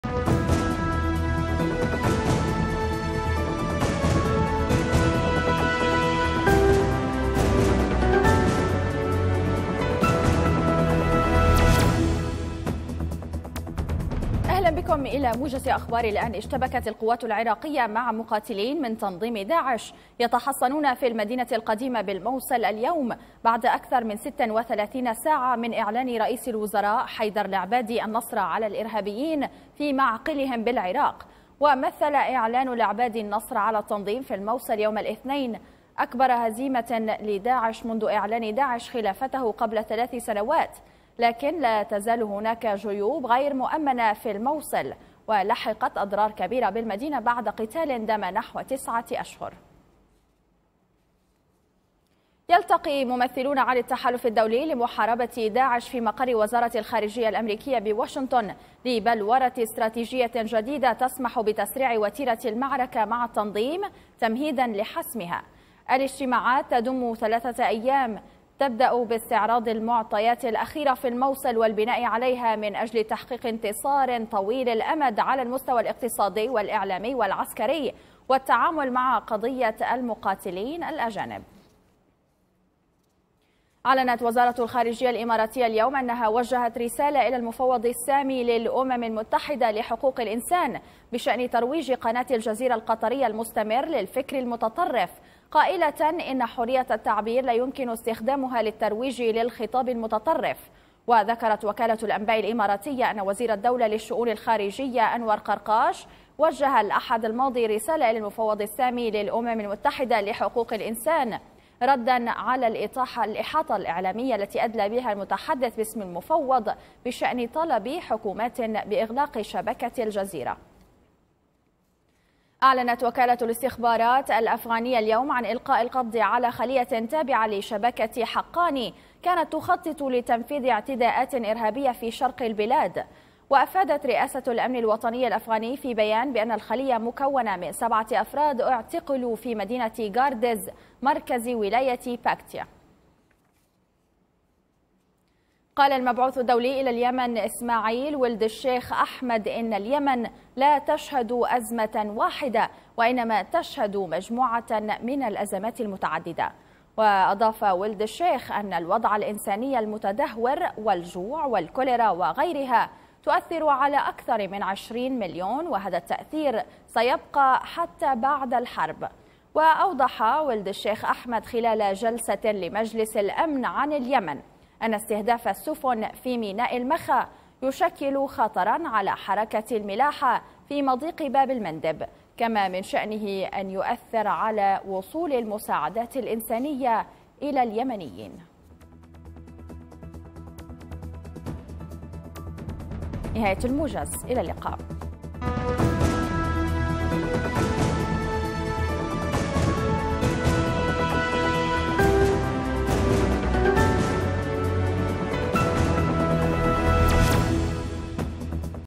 12-07-2017 | موجز الحادية عشر مساءً لأهم الأخبار من تلفزيون_الآن‎
العنواين:
القوات العراقية تشتبك مع داعش فى الموصل بعد أيام من إعلان النصر
اجتماع للتحالف الدولي بهدف تسريع وتيرة المعركة ضد
وزارة الخارجية توجه رسالة إلى المفوض السامي لحقوق الانسان
الاستخبارات الأفغانية تفكك خلية كانت تخطط لعمليات إرهابية
المبعوث الدولي: اليمن يشهد مجموعة من الأزمات المتعددة